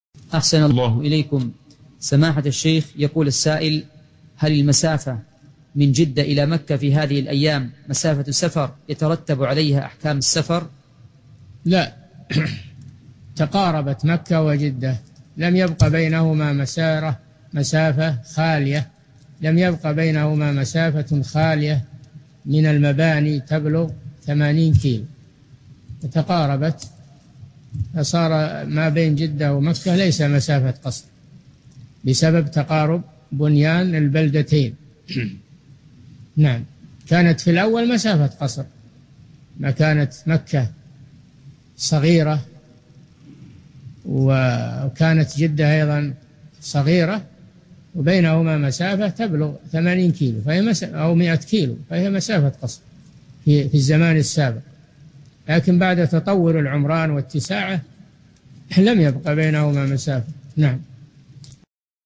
[70 /84] هل المسافة من جدة إلى مكة في هذه الأيام يترتب عليها أحكام السفر؟ | الشيخ صالح الفوزان
[70 /84] السؤال: هل المسافة من جدة إلى مكة في هذه الأيام مسافة سفر يترتب عليها أحكام السفر؟
 الجواب: لا، تقاربت مكة وجدة لم يبق بينهما مسافة خالية من المباني تبلغ ثمانين كيلو، وتقاربت فصار ما بين مكة وجدة ليس مسافة قصر بسبب تقارب بينان البلدتين، كانت في الأول مسافة قصر لما كانت مكة صغيرة وكانت جدة أيضا صغيرة وبينهما مسافة تبلغ ثمانين كيلو أو مائة كيلو فهي مسافة قصر في الزمان السابق، لكن بعد تطور العمران واتساعه لم يبق بينهما مسافة."
السيرة الذاتية :
للاشتراك في قناة الشيخ صالح الفوزان
أسئلة وأجوبة من شرح كتاب السنة من سنن أبي داود (84) مقطع

للاستفسار والإقتراحات والدعم
رئيس الفريق
لمشروع كبار العلماء